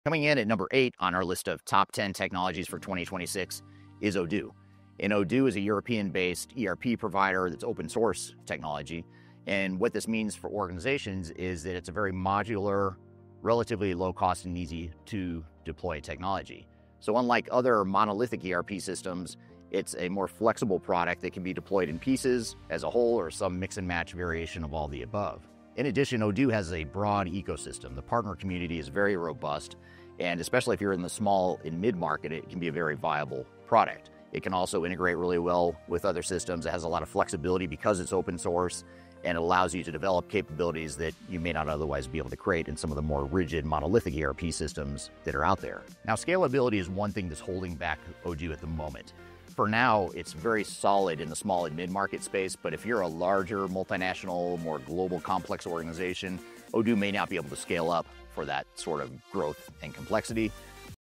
Odoo Is The ERP That's Disrupting Business (But Does It Scale?)
Odoo, a European open-source ERP, offers modular, low-cost deployment, unlike monolithic systems. Its flexible ecosystem and integration make it viable for small and mid-market organizations. Scalability limitations exist for large, complex global organizations.